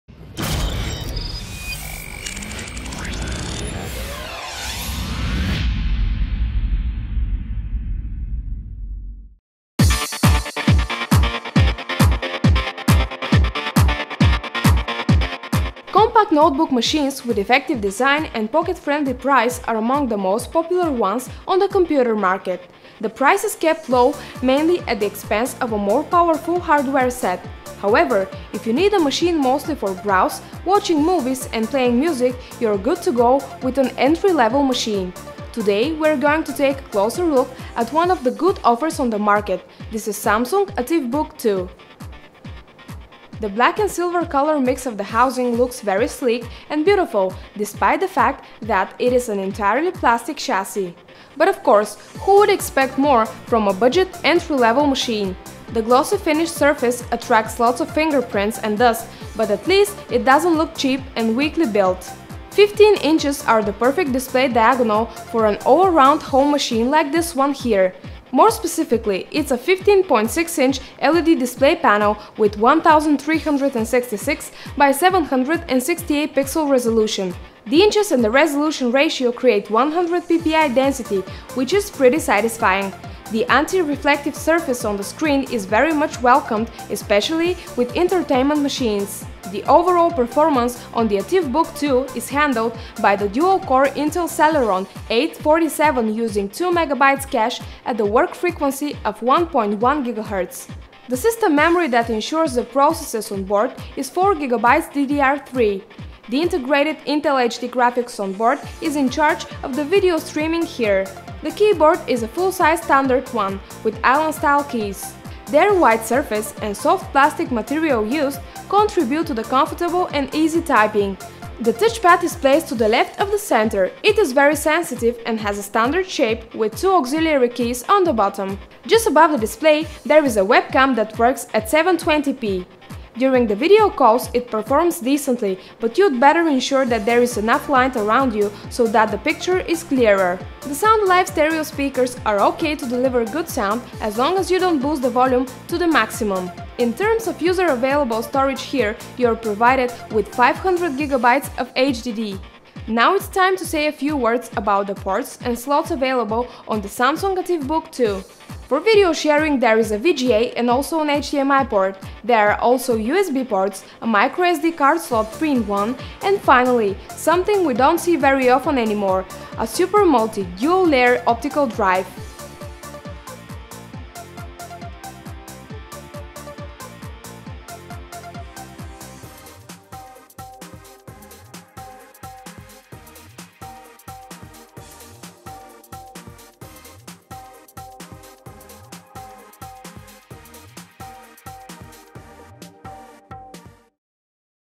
Samsung Ativ Book 2 Is a Stylish Everyday Notebook
Quick Stats
Type: Smartphone

Brand: Samsung

Hardware: 15.6 -- inch LED display, dual-core Intel Celeron 847 (2MB cache) @ 1.10GHz, 4GB DDR3, integrated Intel HD graphics, 500GB HDD storage, VGA port, HDMI ports, microSD card slot 3-in-1, USB ports, super Multi dual layer optical drive, 720p webcam

Technology: Windows 8, LED-backlit, 720p, Sound Alive stereo speakers, 100ppi display pixel density, anti-reflective surface, 1366x768 pixel resolution,

What We Liked:
price

What We Disliked:
Sluggish CPU performance

What We Thought

If you need a machine mostly for browse, watching movies and playing music you are good to go with an entry-level machine